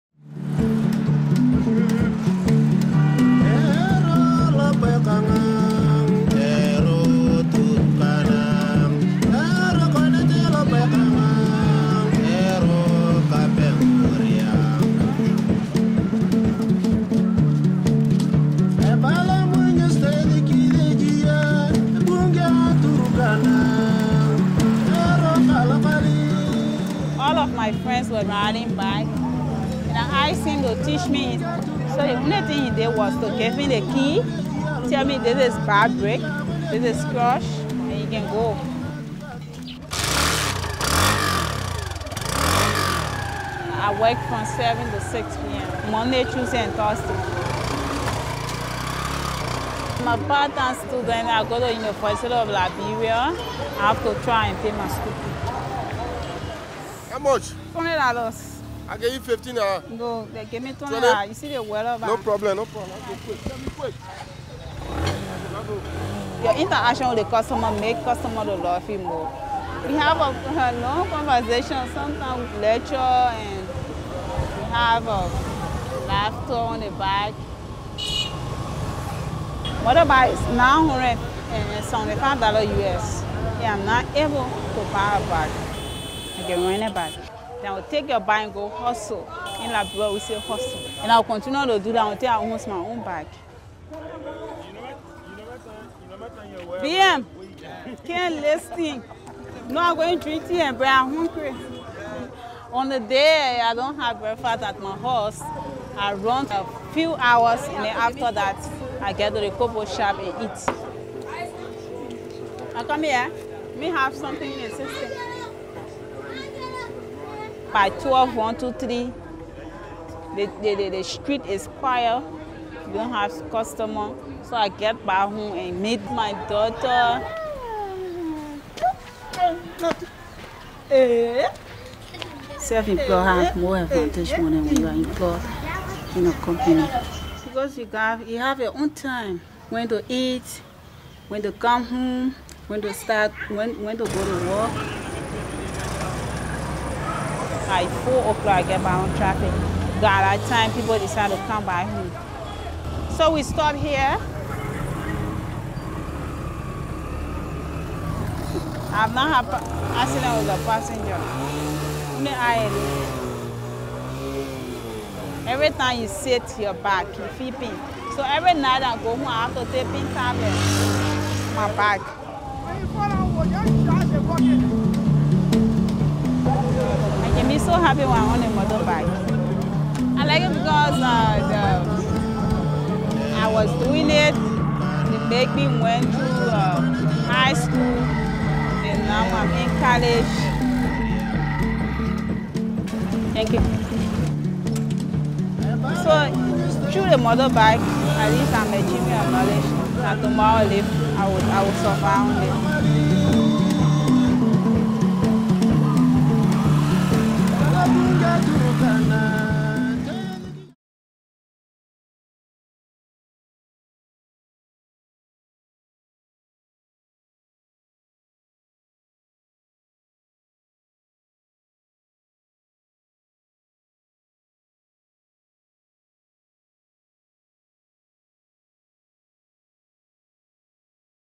A Working Day – Taxi Driver, Liberia
A new series of films by The School of Life about work around the world - a guide to helping us to think about what makes work purposeful and worthwhile.

CREDITS